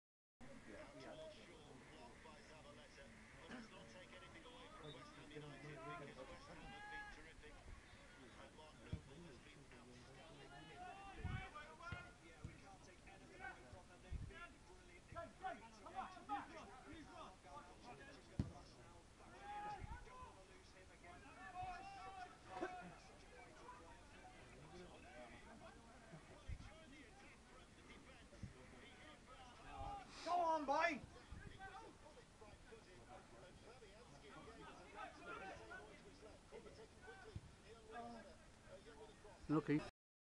CCFA and Bond Timber Junior Cup 1st Round Game ,St Stephen Reserves 2 v 4 Lostwithiel 1st team
Stuart Kesson Smith shoots wide for St Stephen Reserves.